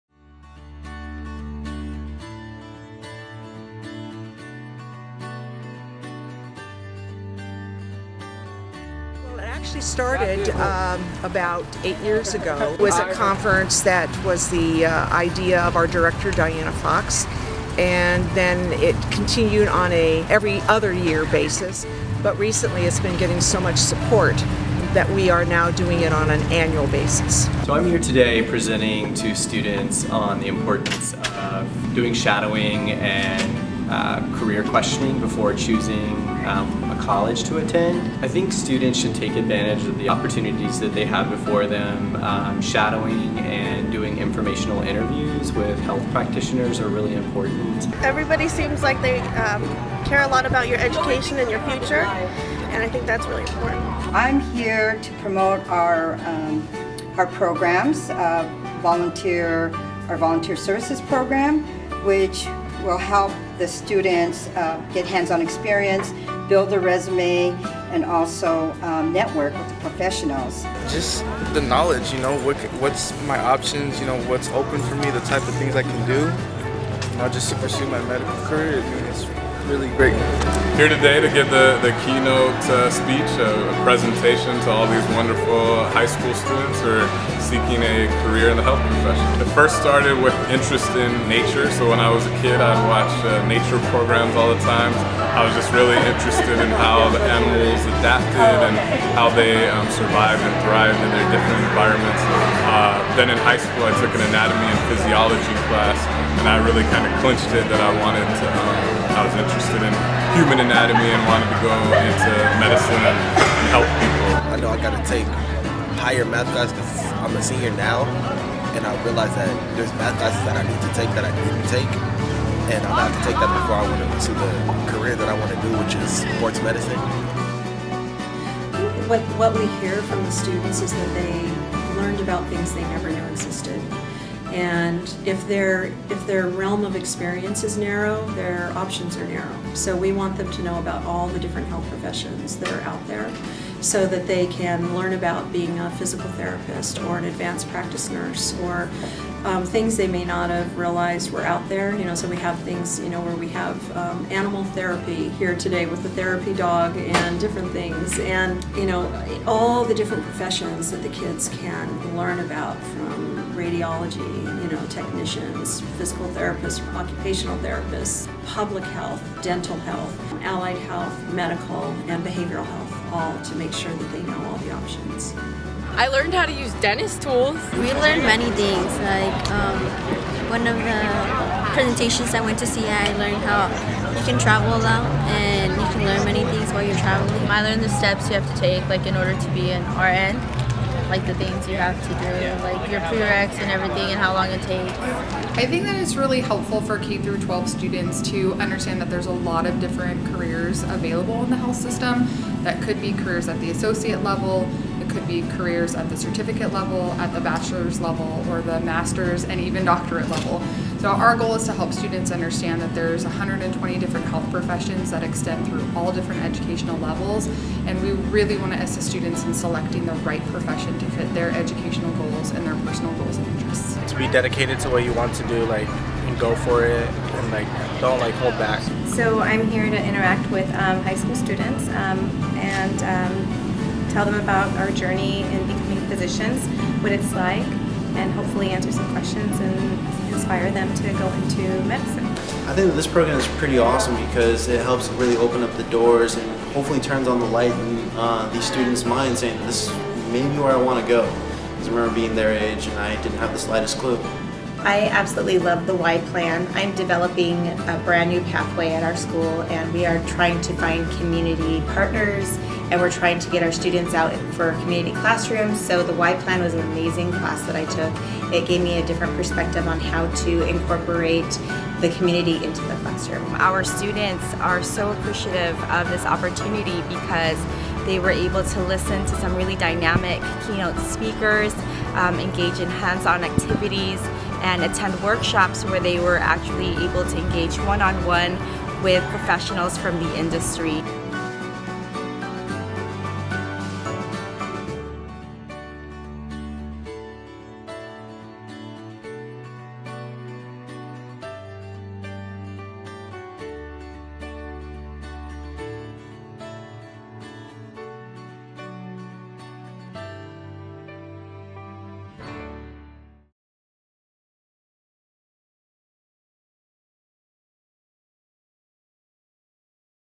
Inland Coalition Health Professions Conference 2016
A one day conference, created by the Inland coalition, to teach high school students about different health professions.